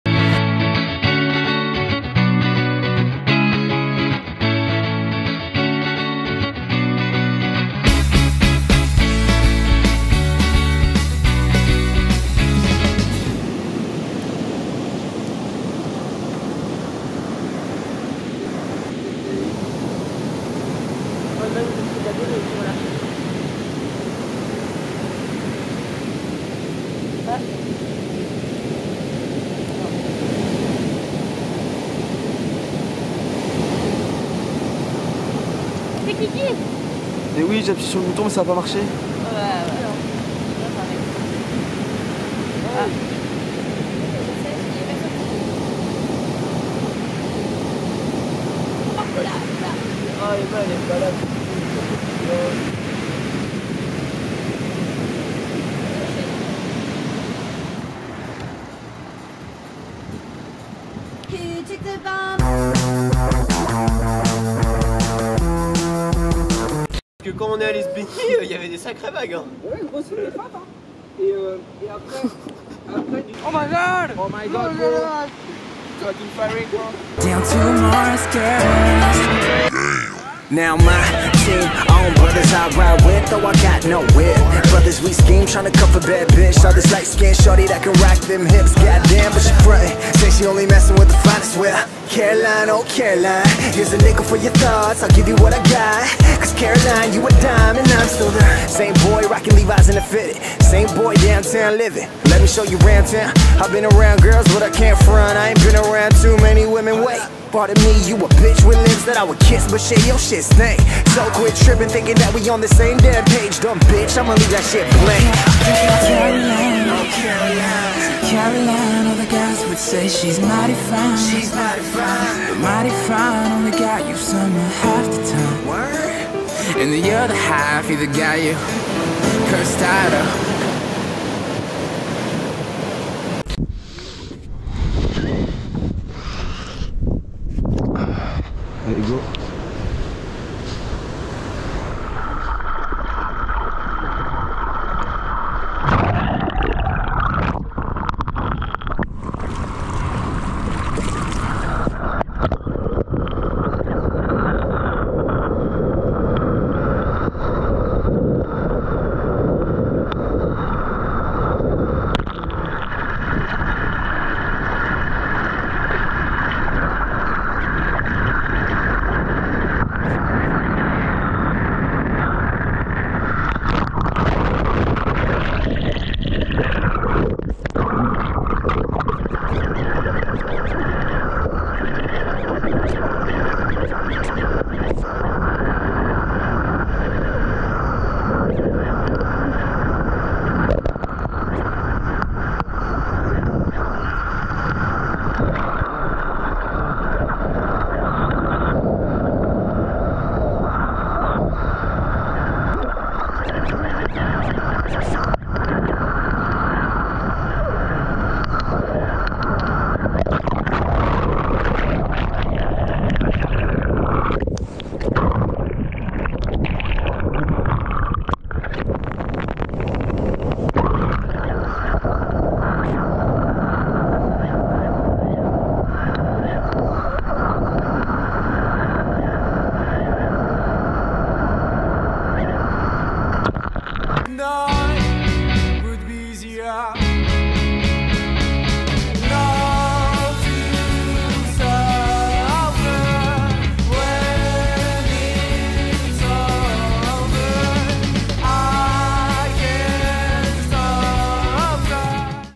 MEILLEURS SESSIONS DE CET ÉTÉ ? SURF DE NUIT AVEC LES COPAINS 💥 | RAW surf & POV Killian M
salut à toi ! j’espère que cette nouvelle vidéo récap de mes meilleurs (et pas nombreuses) sessions de cet été t'a plu, si c'est le cas n’hésite pas à me le faire savoir en commentaire ! merci et moi je te dis à la prochaine à l'eau !

Merci à @lil_portter pour les vidéos, à @photorider85 pour la photo de la miniature 🙌

Mes partenaires :

                        MES COMBINAISONS :
-15%  sur tout le site avec mon code : KILLIANM

                               SALTYSMILE :
-20% avec mon code SALTY_KILLIAN